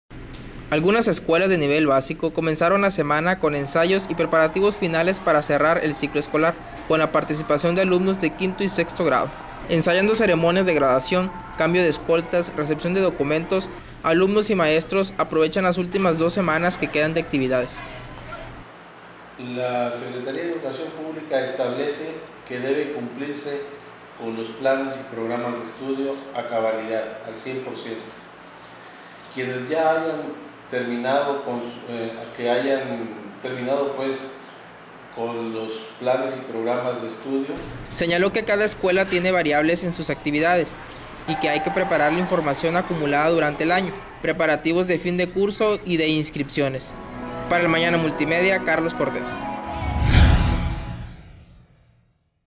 Dedican escuelas últimos días a eventos
La mayoría tranaja en preparativos de graduación
Información e imágenes: Carlos Cortez

El Mañana de Reynosa.